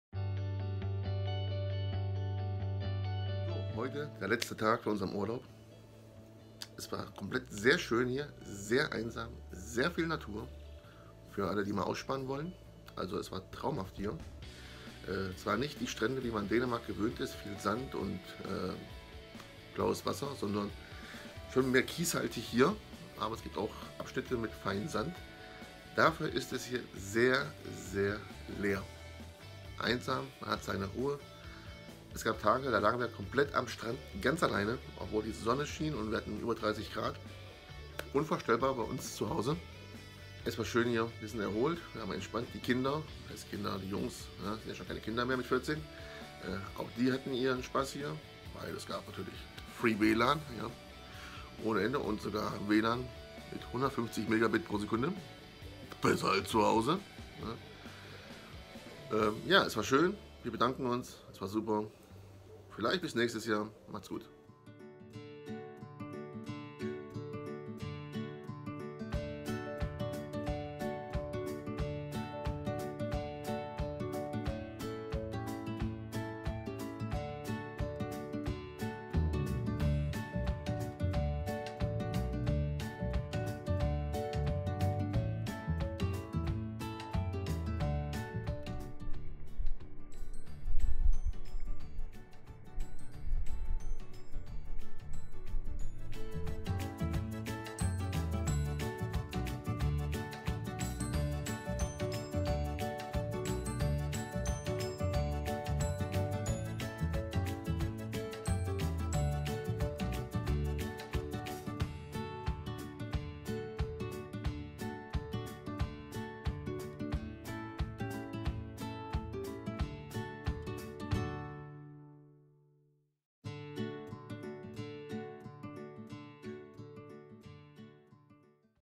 Dänemark 2018 - Urlaub am Limfjord
Unser Urlaub am Limfjord in Dänemark im Sommer 2018.

Schaut auch mal bei Marc Schmidt rein.